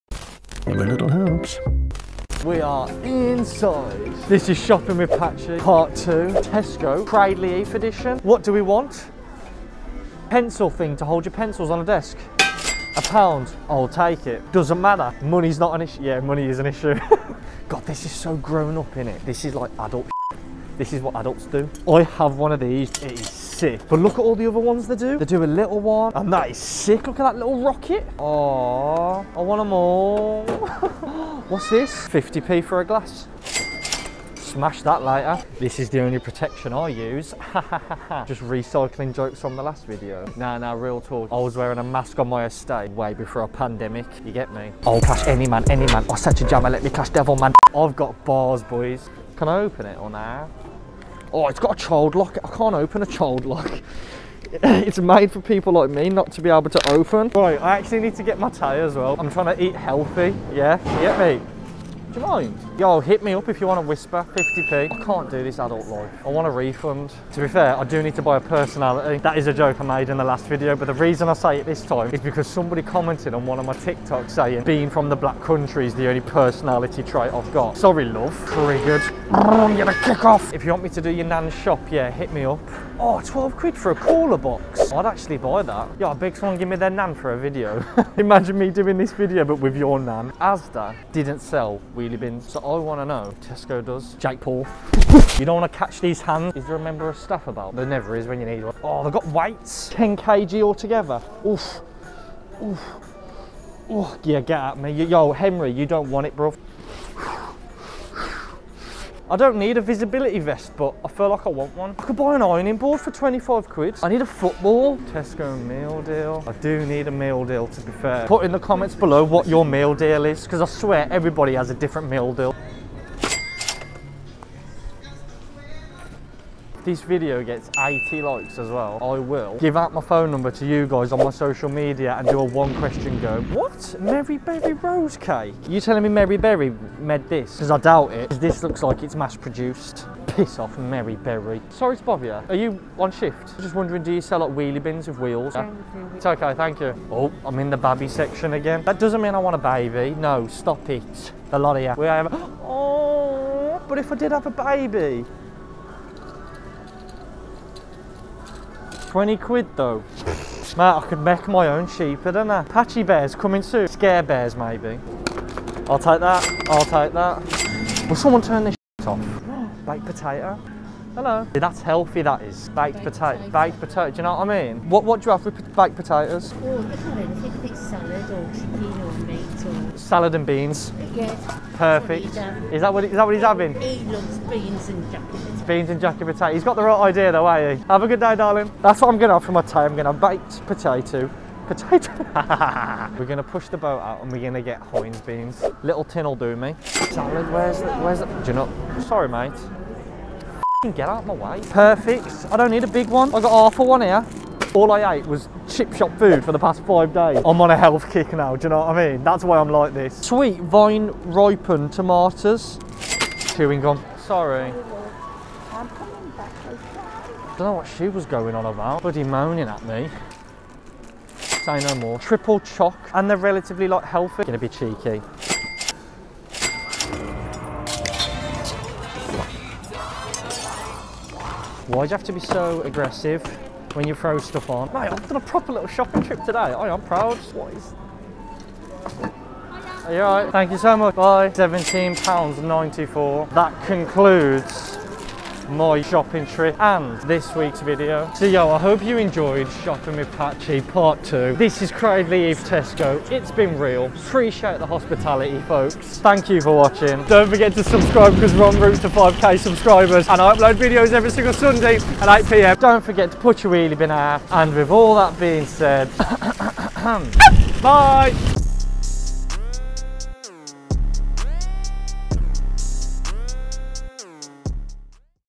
Shopping with patchy Cradley Heath edition
this video is part 2 in the shopping with patchy series first video is linked at the end of this one hope you enjoy if so let me know in the comments below!